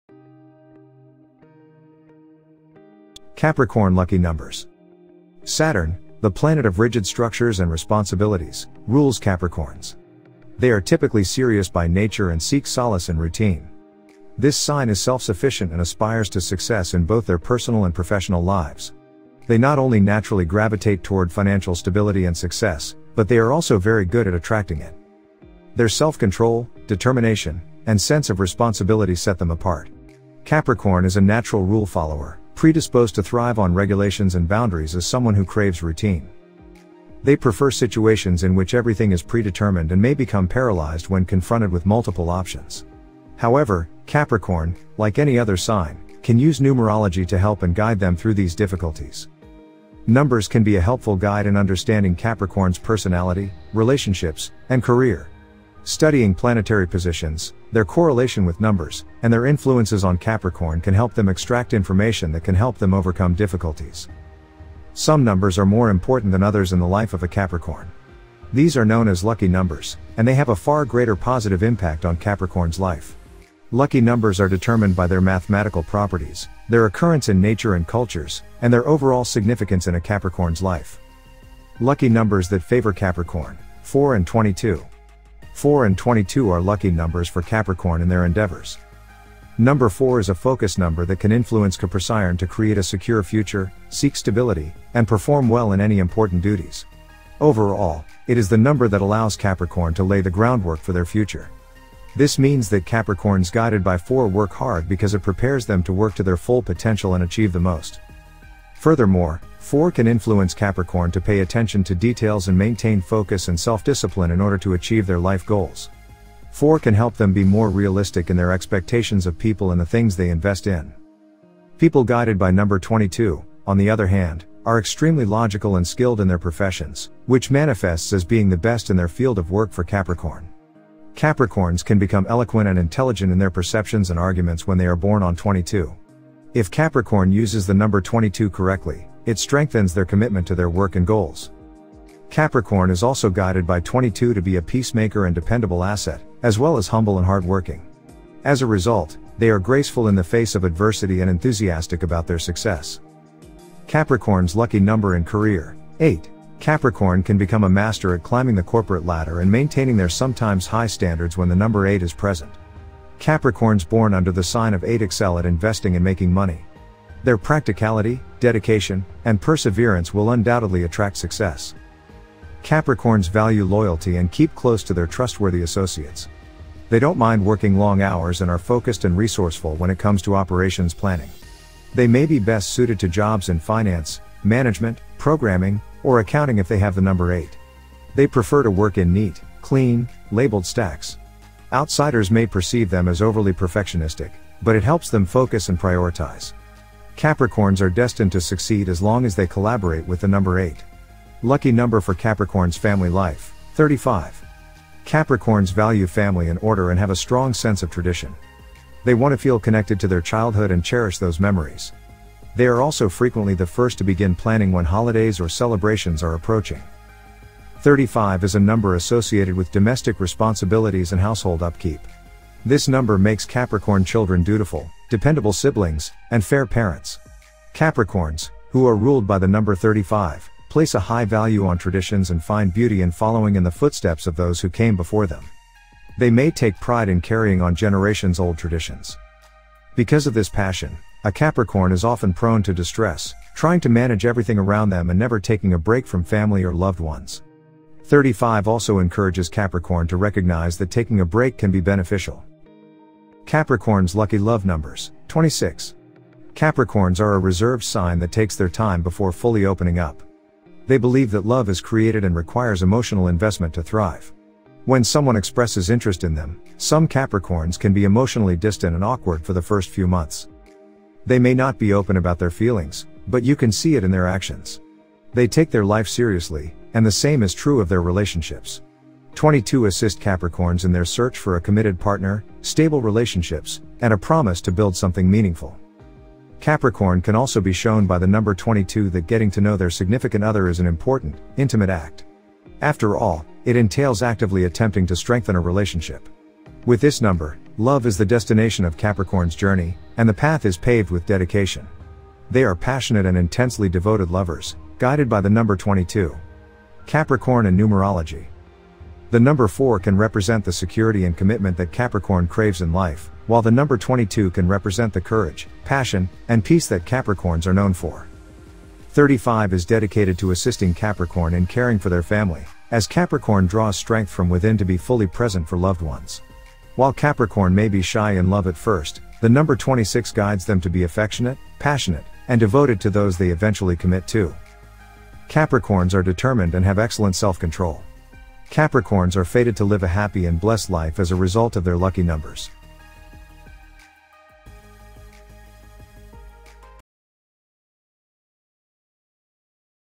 Capricorn Lucky Numbers
Lucky Numbers That Favor Capricorn: 4 and 22.
Capricorn’s Lucky Number in Career: 8.
Lucky Number for Capricorn’s Family Life: 35.
Capricorn’s Lucky Love Numbers: 26.
Capricorn & Numerology.